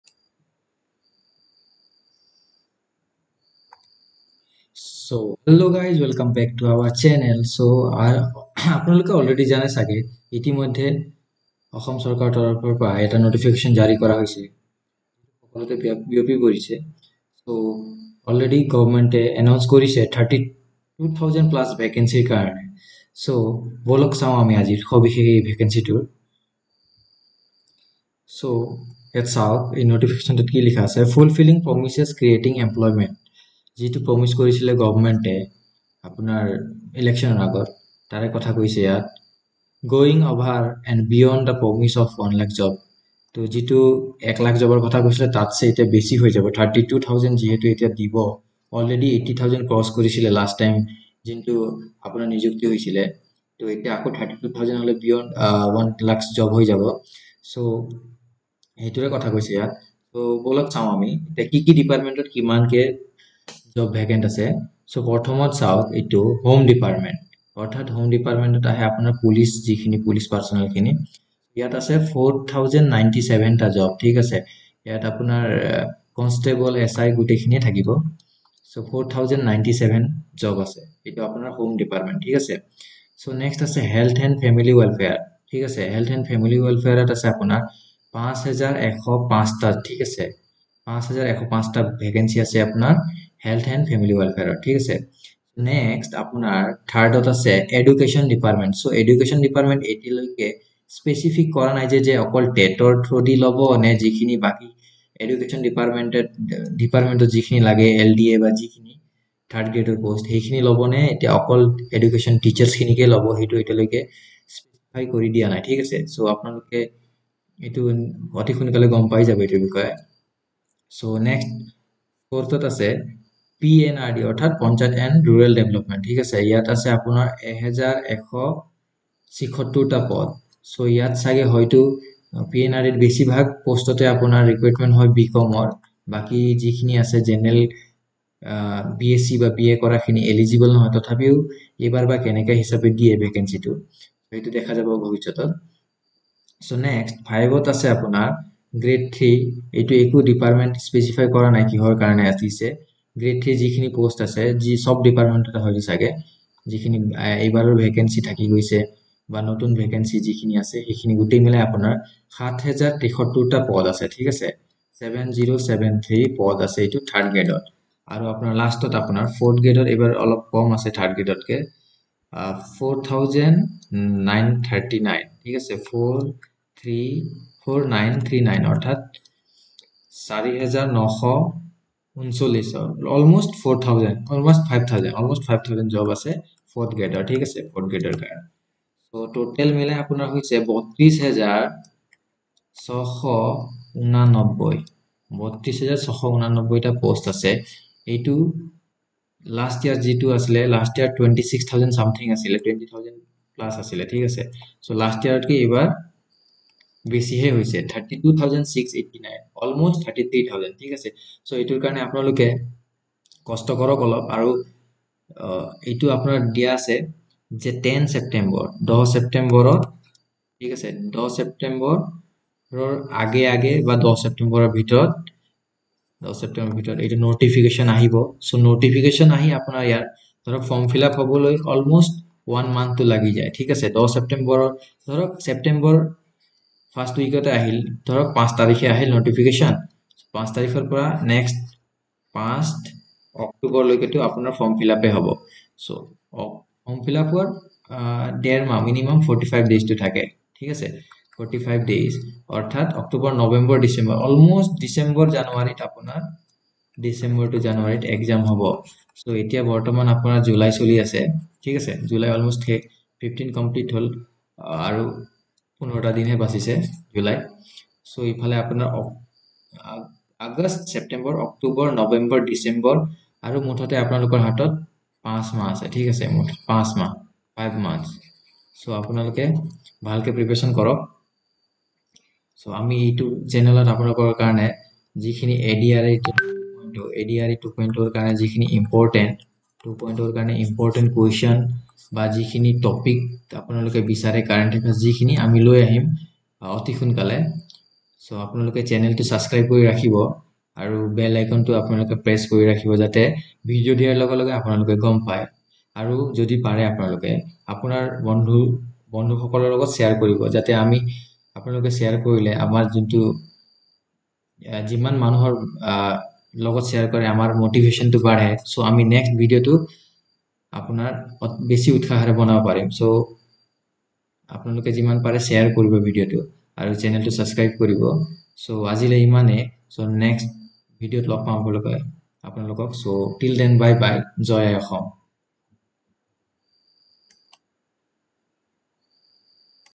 ADRE 2.0 NEW NOTIFICATION 2023
In this video I am disussing about adre 2023 vacancy details